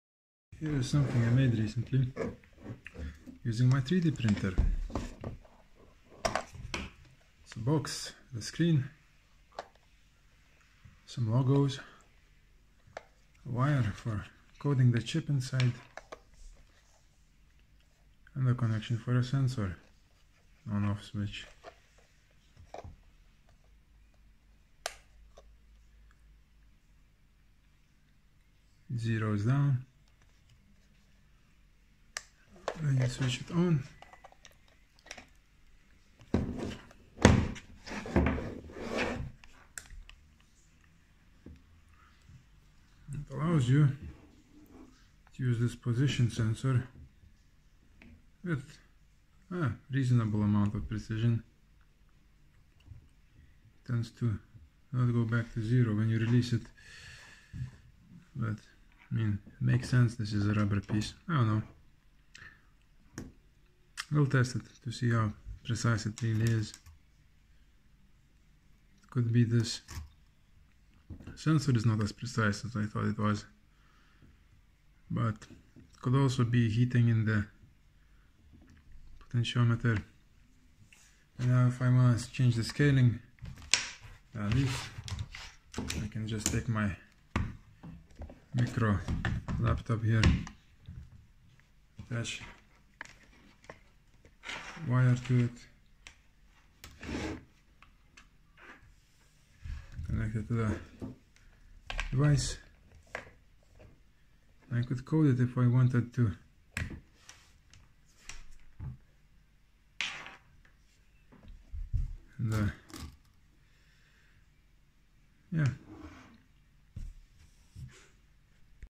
3D printer enclosure - Arduino sensor, LCD
Using CreaBot F430 I printed an ASA enclosure.
Used HIPS supports. Lemonine melting the holes or the screws...
Could make a more detailed video on how to get the machine running, the settings and post processing of the print. Also schematics of the Arduino wiring, AD1110, LCD and so forth... put requests into comments. So I know what to film, mention, describe as I experiment with the hobby for you.